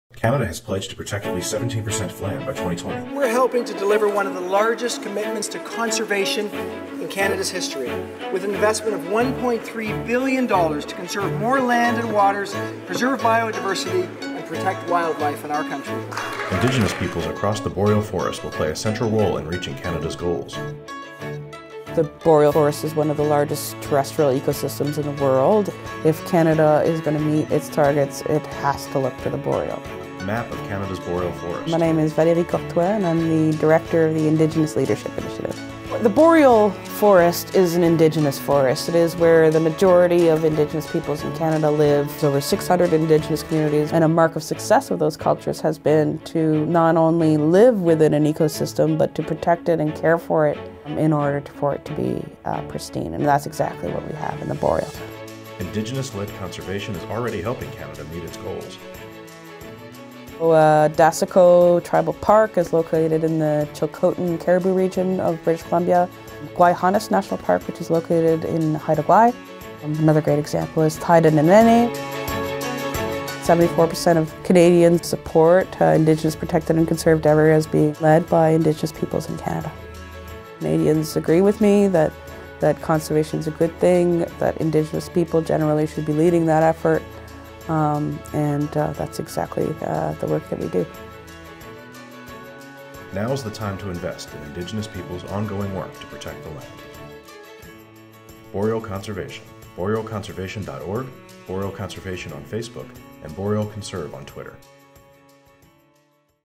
Now is the Time for Indigenous-Led Conservation in Canada (Audio Described)
Canada has pledged to protect at least 17% of land by 2020, and Indigenous-led conservation is already helping Canada reach its goal.